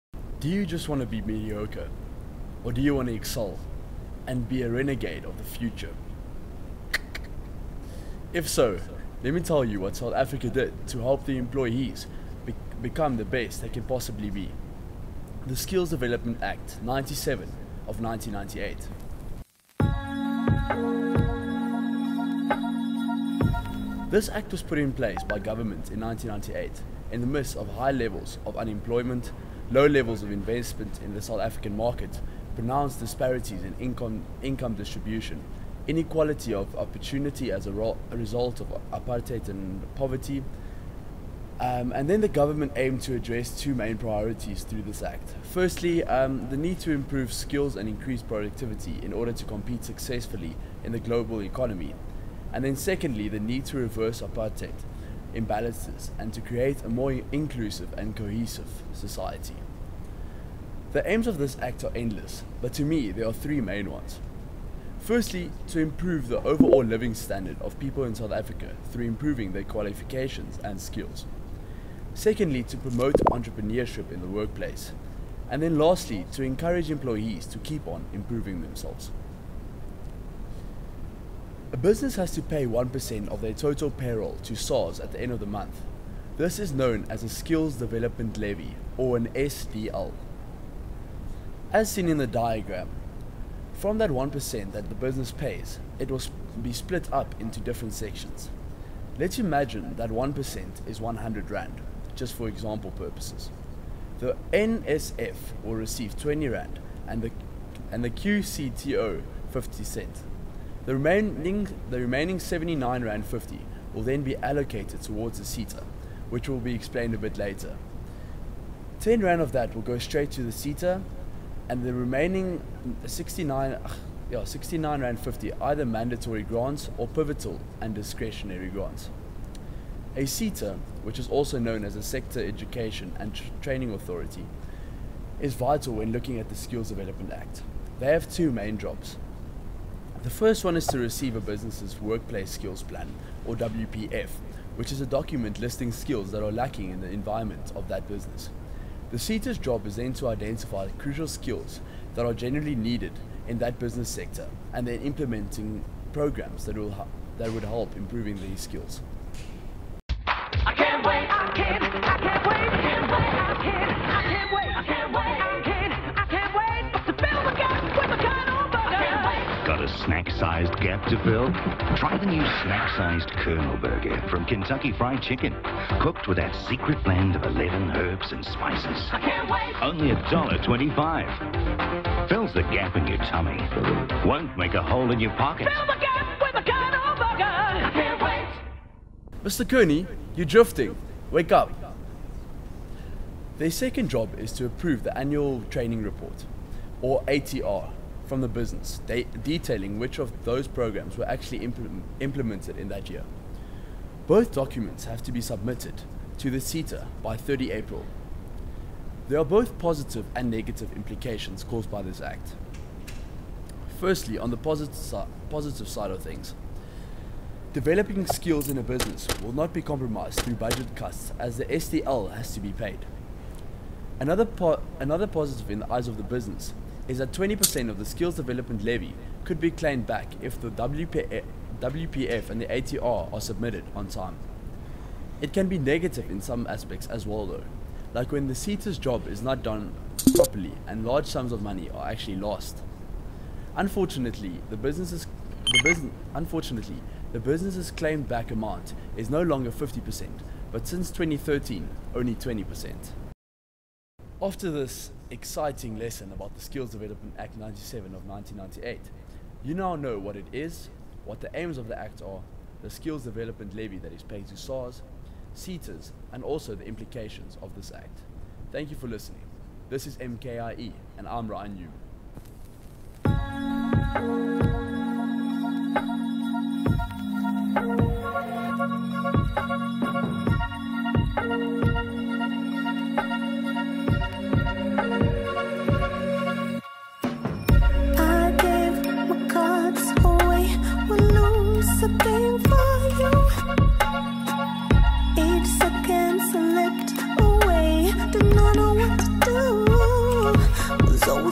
The Skills Development Act - MKIE Lesson 11
Mark Kearney’s Institute of Excellence
Chapter 4 Summary (Video lesson form)

Edited and Compiled by Keroshan Pillay.

Scripted by: Rein Hulme